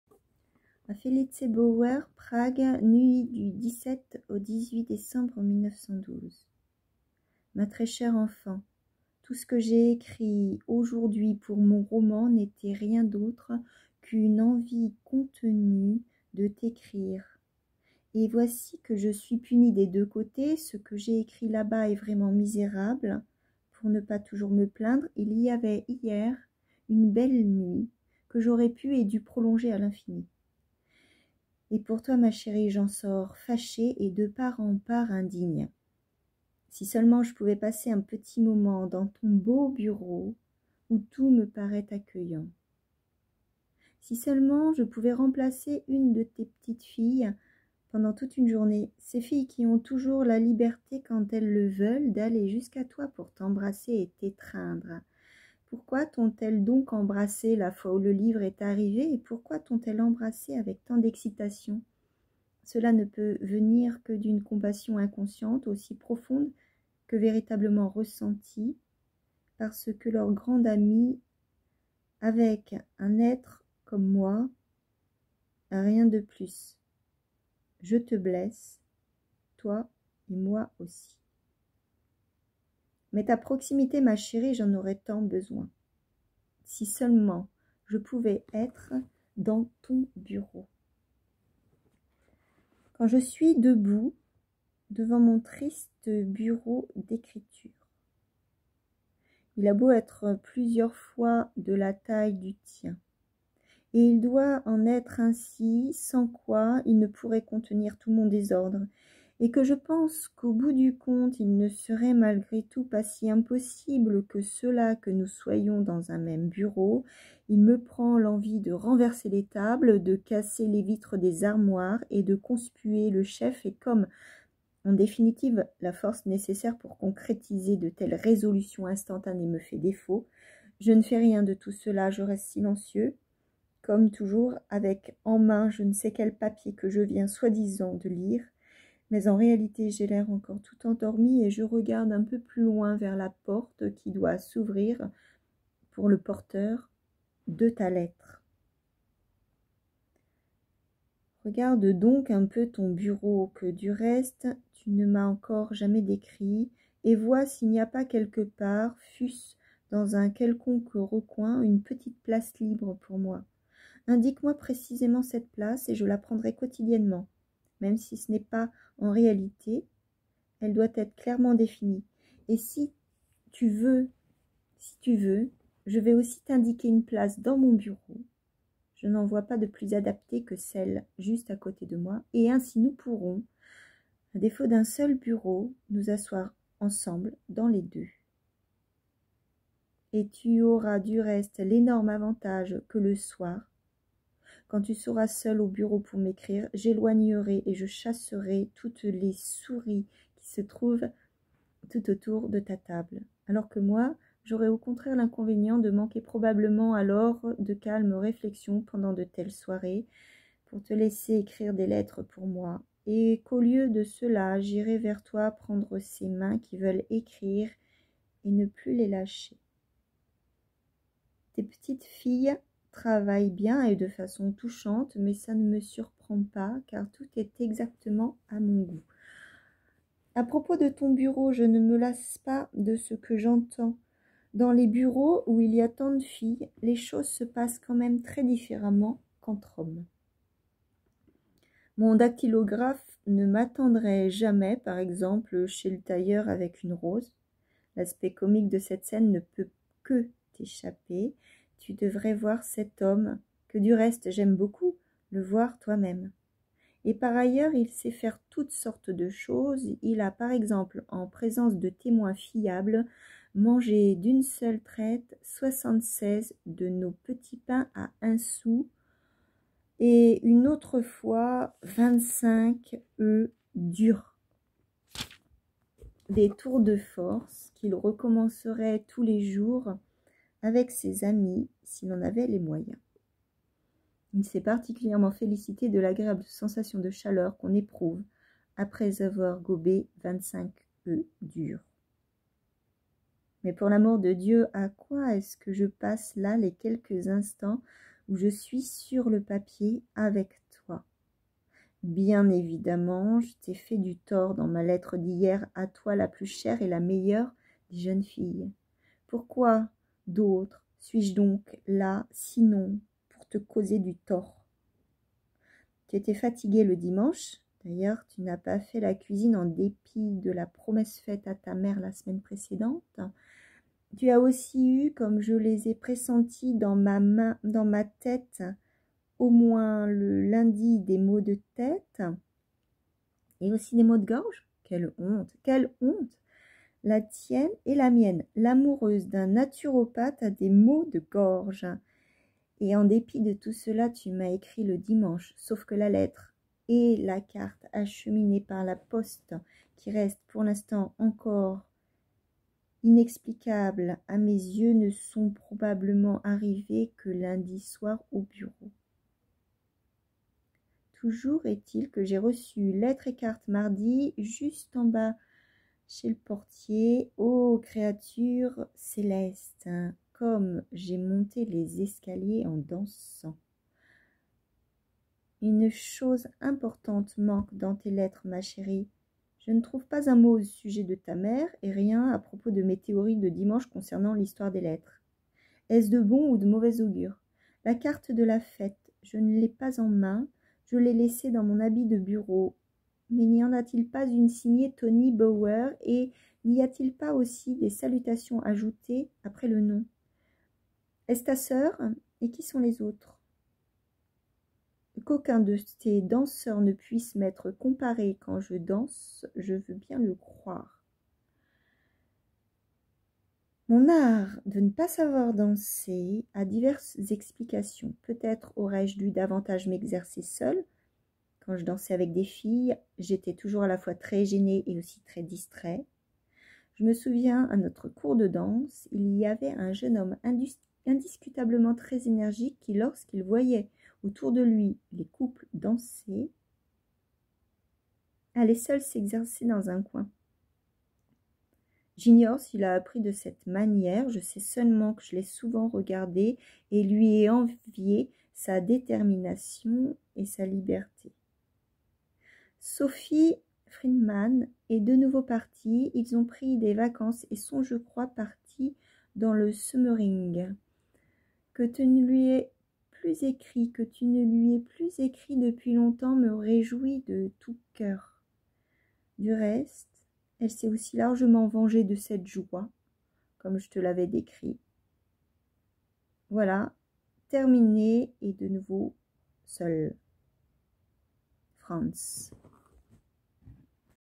86ème lettre de Franz Kafka à Felice Bauer, nuit du 17 au 18 décembre 1912
Nuit 17 au 18 décembre 1912
Où l'on apprend que Franz ne sait pas danser. Felice danse beaucoup.  (Elle ne sait pas nager).
Que son dactylographe est capable de gober 25 œufs durs à la suite.
Qu'il raconte n'importe quoi (mais pas à nimporte qui) et le regrette : "Mais pour l'amour de Dieu, à quoi est-ce que je passe là les quelques instants où je suis sur le papier avec toi!"

traduction Laure Bernardi et Jean-Claude Rambach éditions Gallimard 22